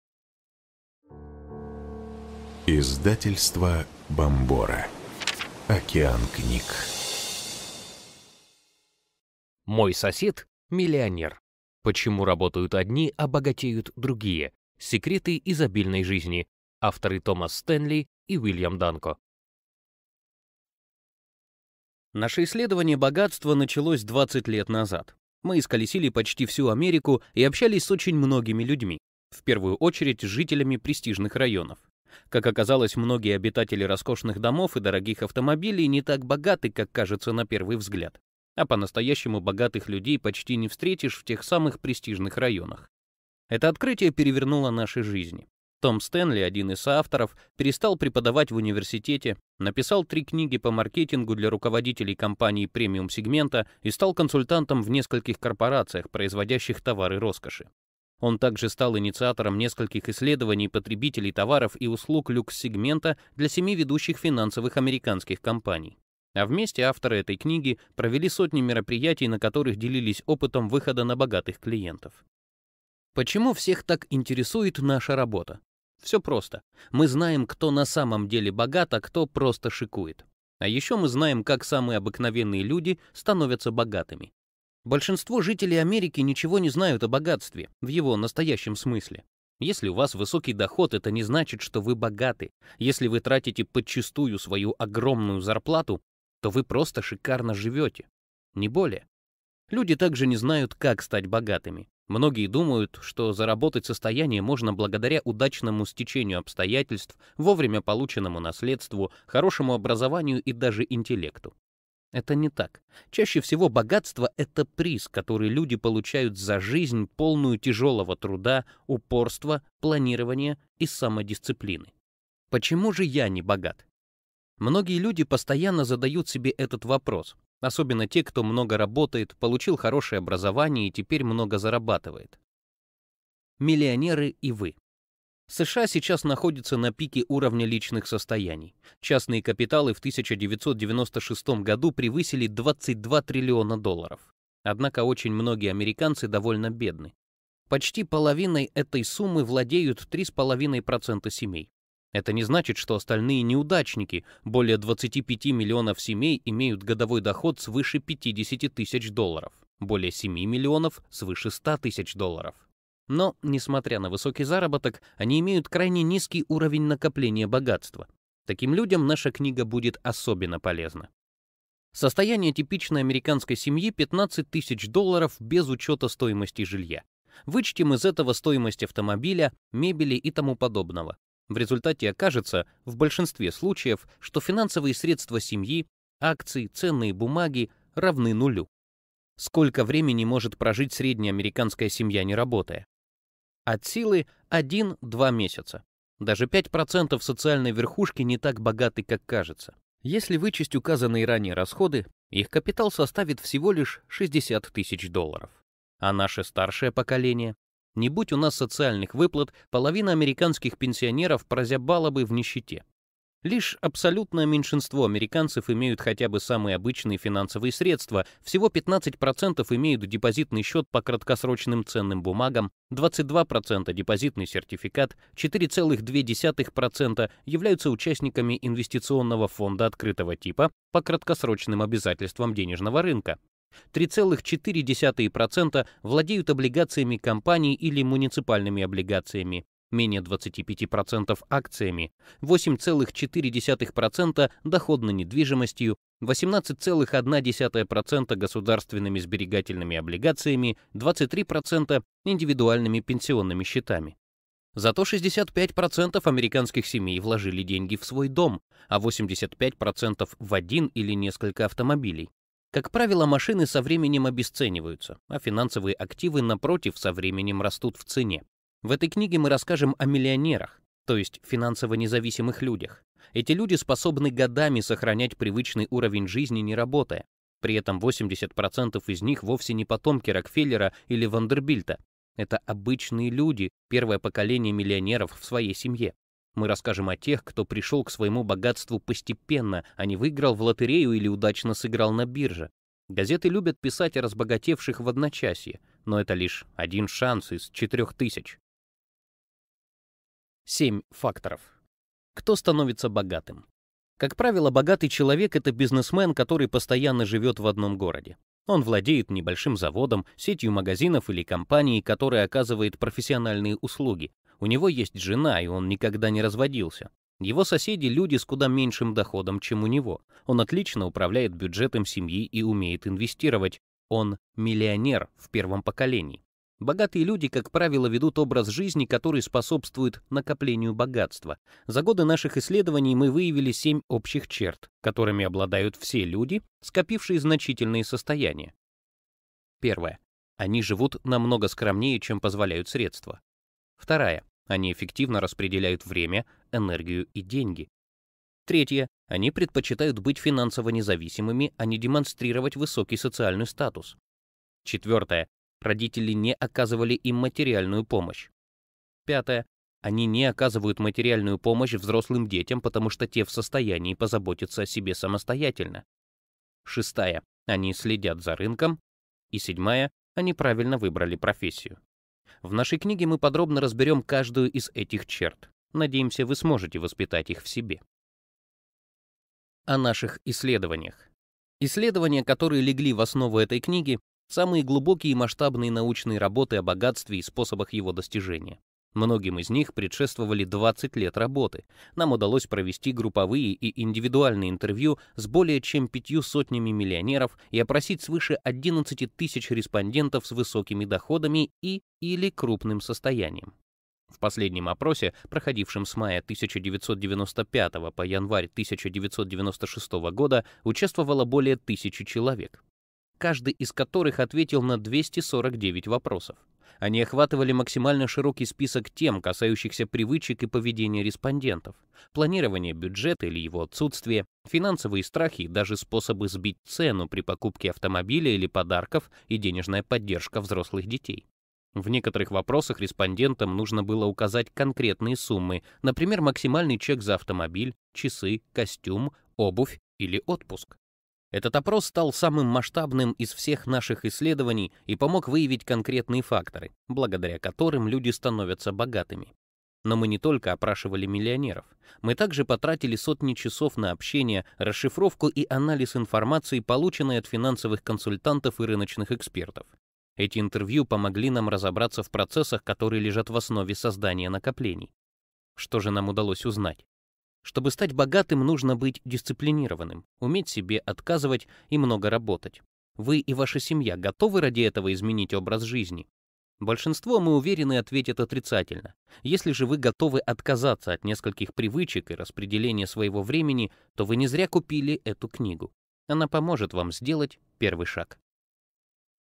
Мой сосед – миллионер. Почему работают одни, а богатеют другие? Секреты изобильной жизни.
Аудиокнига Мой сосед – миллионер. Почему работают одни, а богатеют другие? Секреты изобильной жизни Уильяма Д. Данко – слушать онлайн.
Авторы: Уильям Д. Данко, Ю. Сашникова, Бомбора, Томас Дж. Стэнли.
Жанры: зарубежная деловая литература, личная эффективность, личные финансы
Дата выхода на ЛитРес: 16 марта 2021
Дата написания: 2010
Чтец: Сергей Соколов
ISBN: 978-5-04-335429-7

Описание: Ежедневно тысячи людей задаются вопросом «почему же я не богат?» Казалось бы, хорошее образование, работа 24/7, стабильный заработок, а денег все нет… Чтобы узнать секрет обогащения, авторы книги исколесили почти всю Америку, опросив более 500 миллионеров и свыше 11 000 респондентов с высокими доходами. Это исследование началось более 20 лет назад, но принципы финансового процветания работают до сих пор. Если вам интересно, как самые обыкновенные люди становятся богатыми – эта книга для вас! Эта книга содержит таблицы, графики и иллюстрации в виде ПДФ-файла, который вы можете скачать на странице аудиокниги на сайте после её покупки.